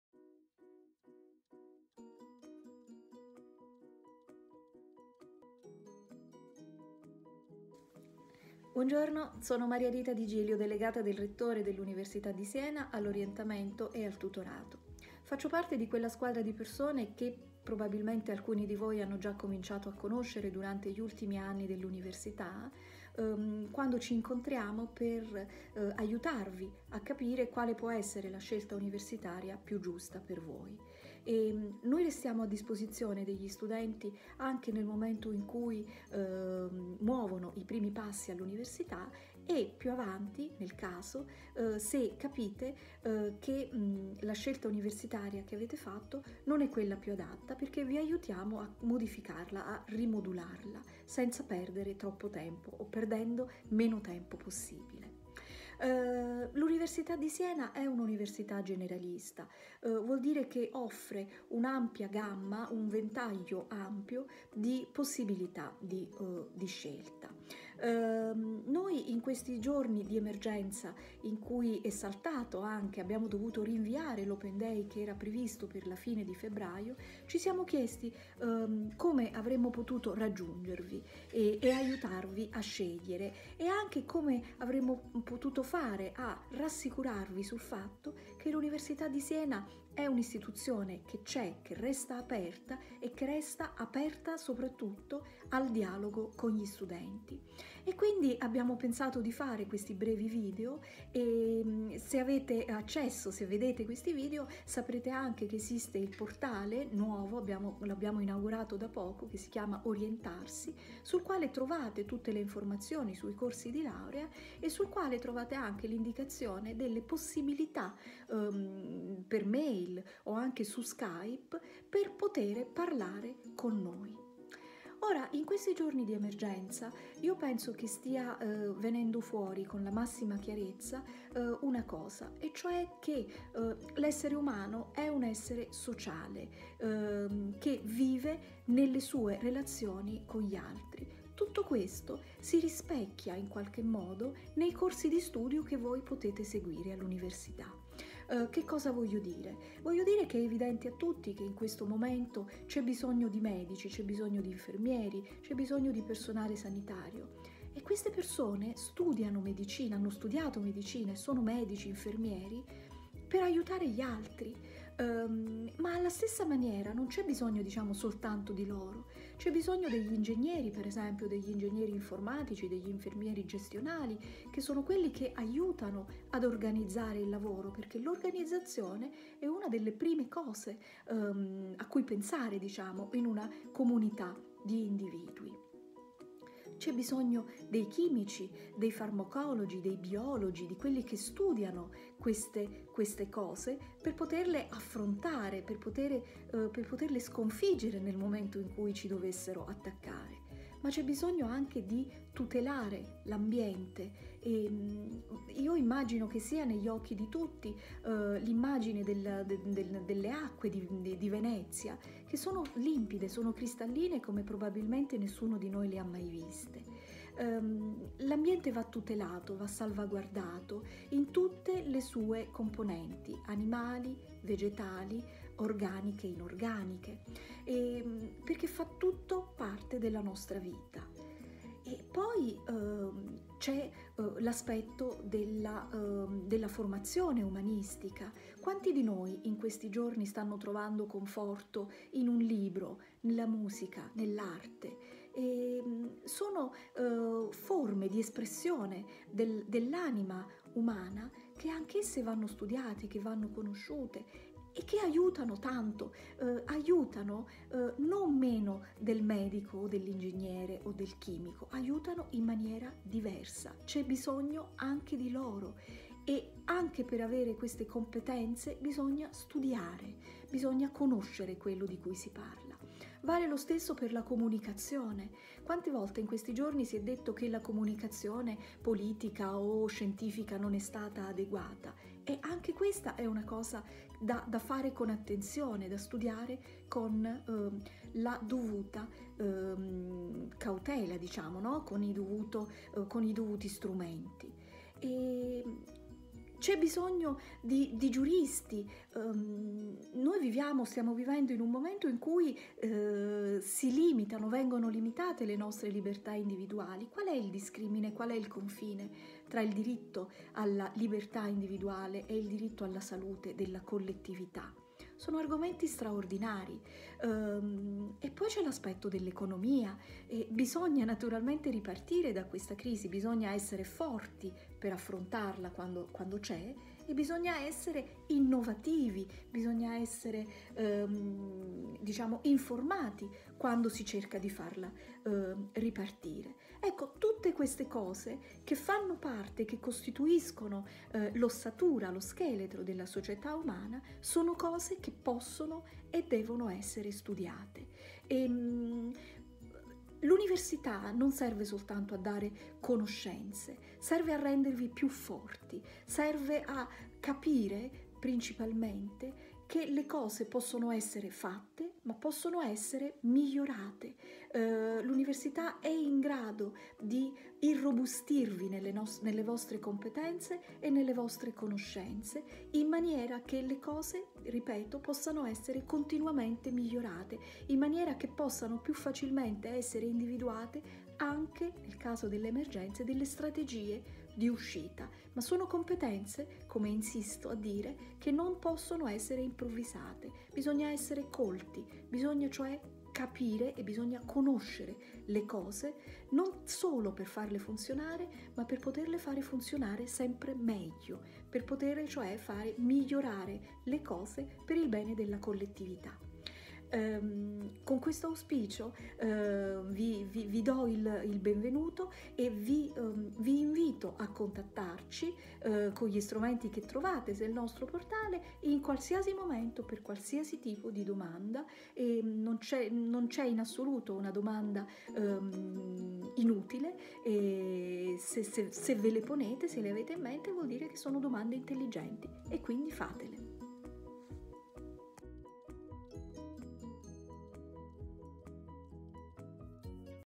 Perché studiare all'Università? Maria Rita Digilio, delegata all'Orientamento e il tutorato
"Questi brevi video sono uno dei modi che, in questo periodo di emergenza, abbiamo pensato per dirvi che l'Università di Siena c'è e resta aperta al dialogo con voi"